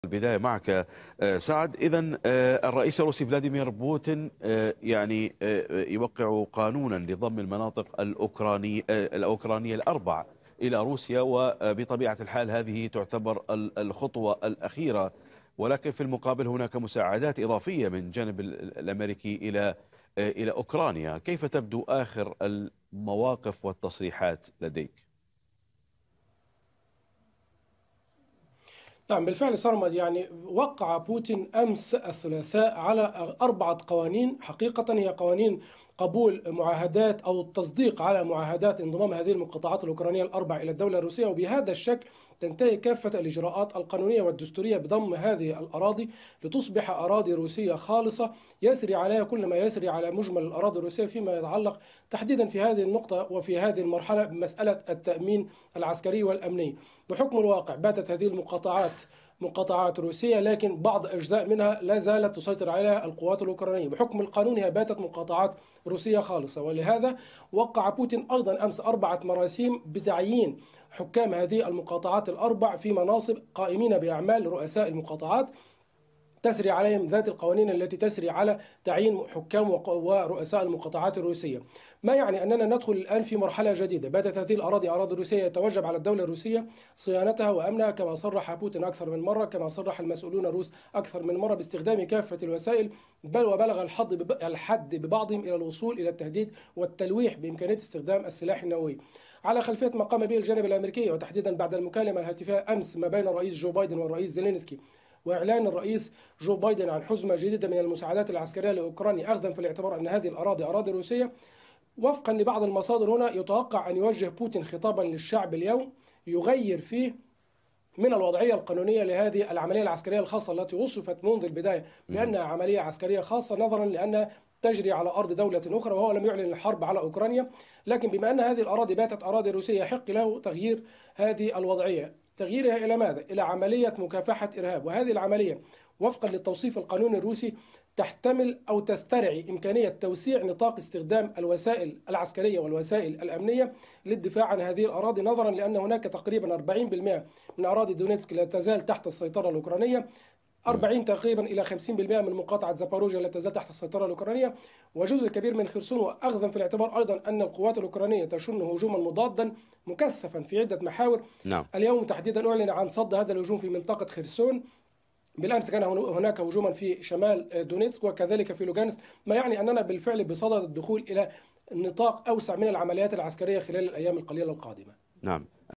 هل يغير بوتين استراتيجيته العسكرية بعد ضم أربع مناطق أوكرانية؟
وقع الرئيس الروسي فلاديمير بوتين قانونا لضم أربع مناطق أوكرانية إلى روسيا، كما وقع مراسيم لتعيين حكام ورؤساء هذه المناطق، في الوقت الذي توقع فيه مراقبون أن يغير الرئيس الروسي الوضع القانوني للعملية العسكرية في أوكرانيا.